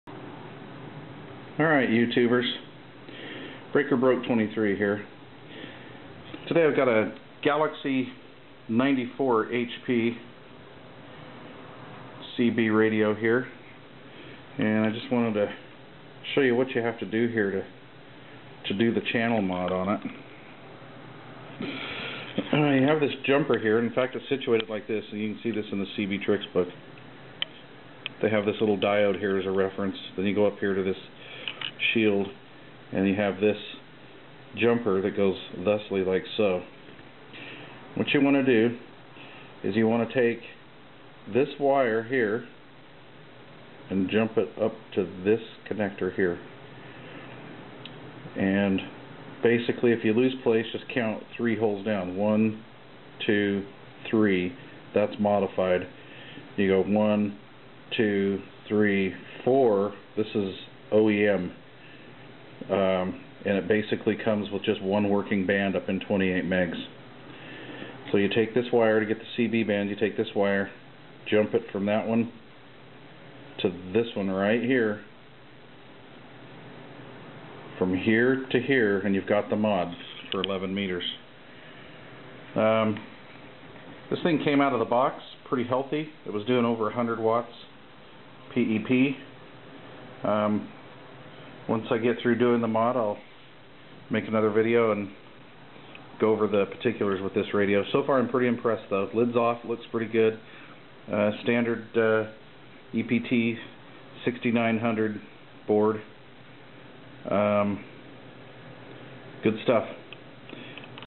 GALAXY DX 94HP DX 98VHP DX95T2 CB CONVERSION
GALAXY DX 98VHP DX 94HP  DX95T2 Convert to CB Radio Band.
This video is intended for experimental use.

Use caution.
Your radio may vary.

This Video was filmed on November 18th 2011